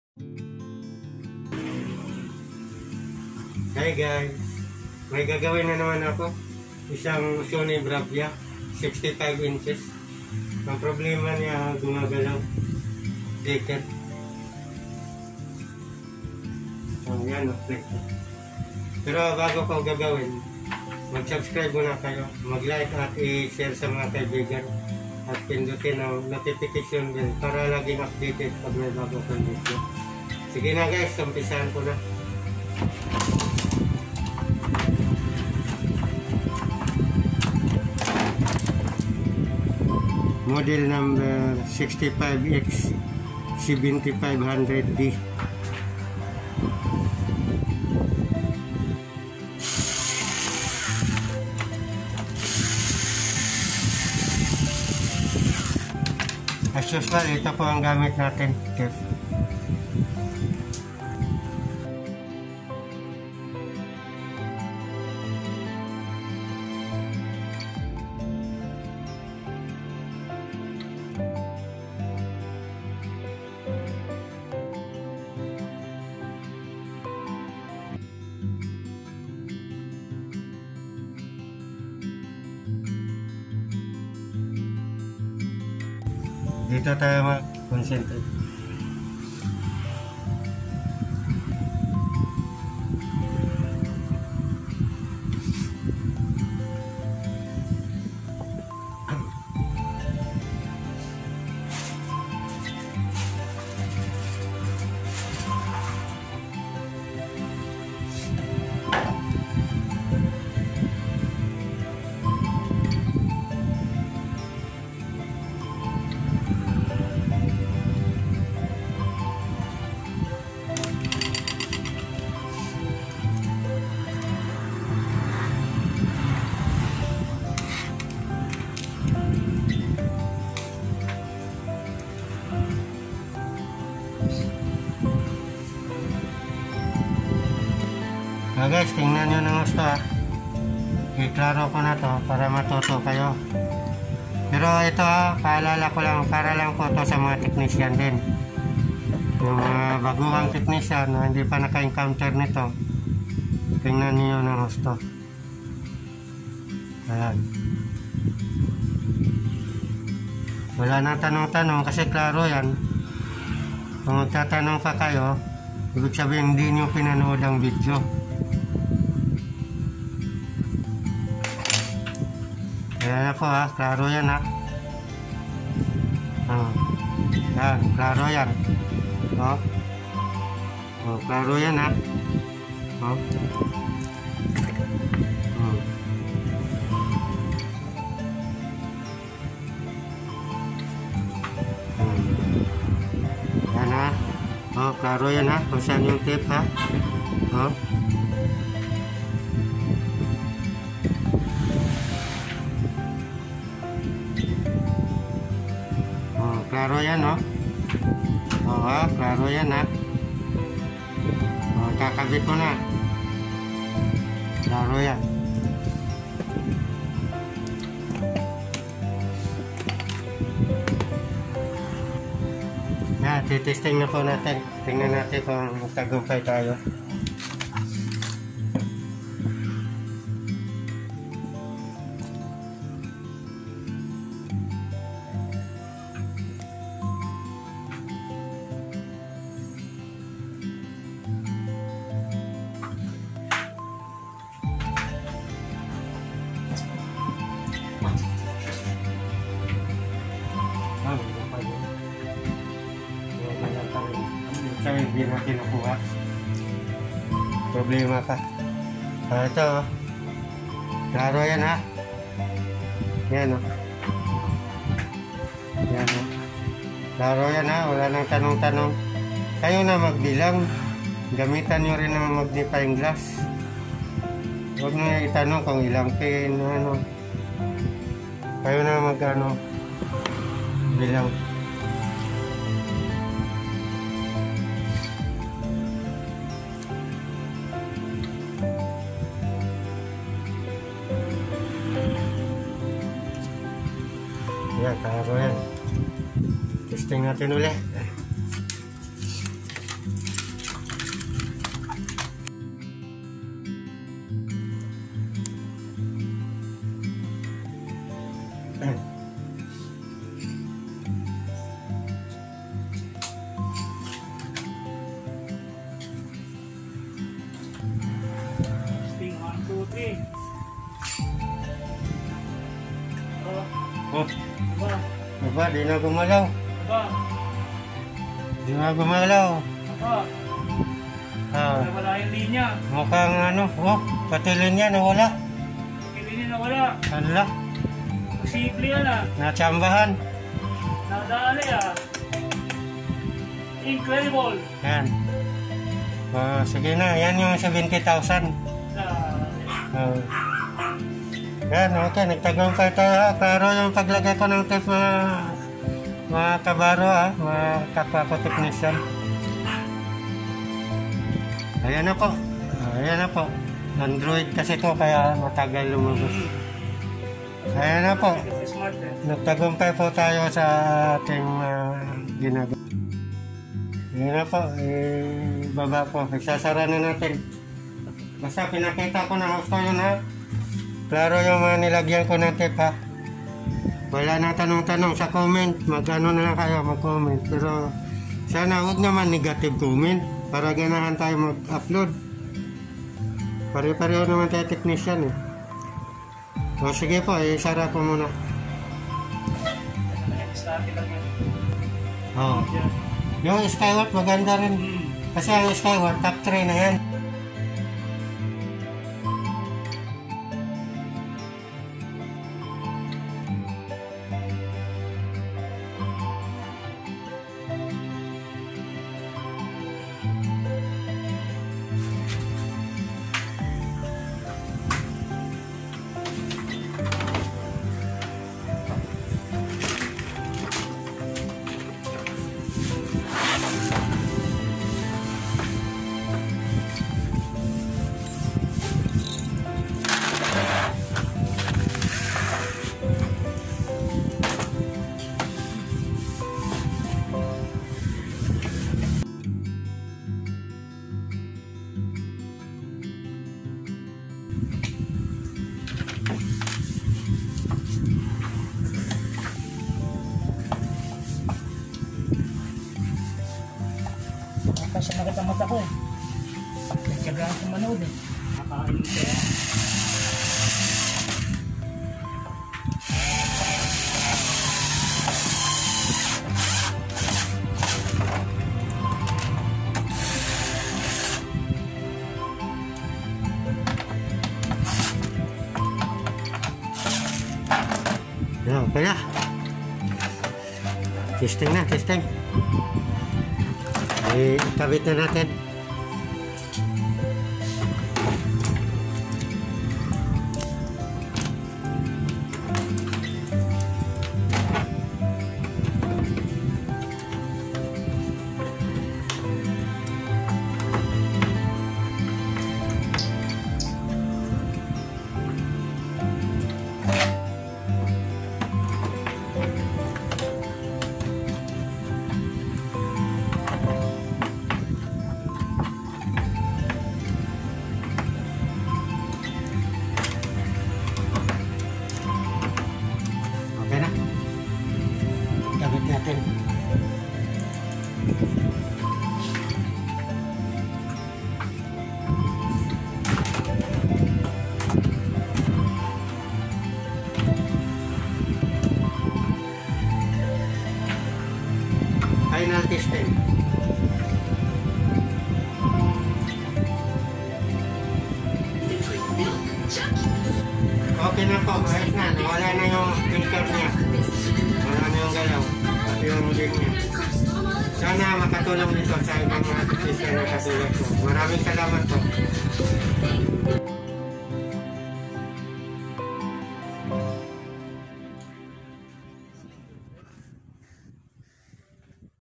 HOW TO FIX FLICKERING DOUBLE IMAGE OF A 65 INCHES LED TV
double image
sony Bravia